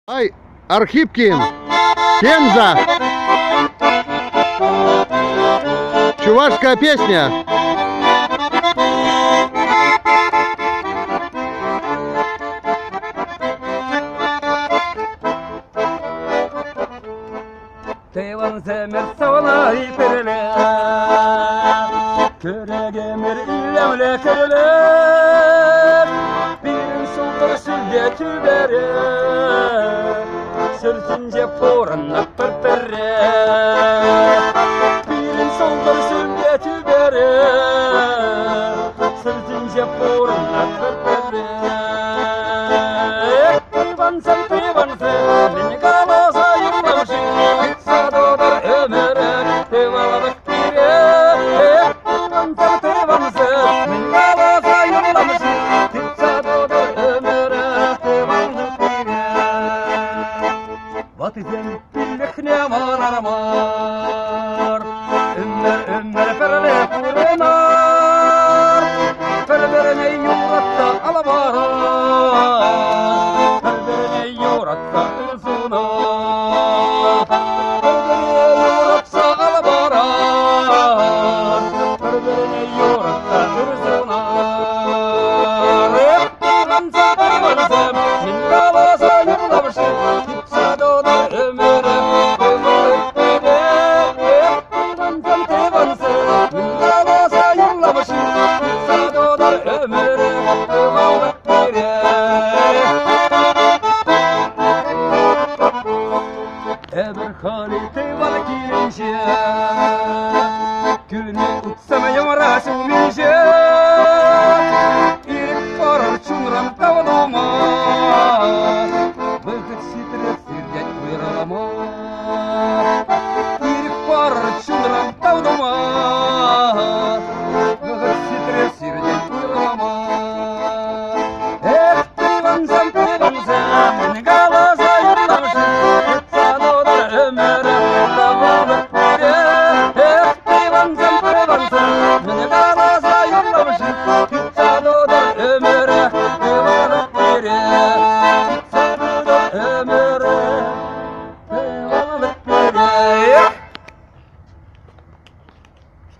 Во даёт Дядя Коля!!!
Заслуженный Гармонист России Николай Архипкин - Пензенская область.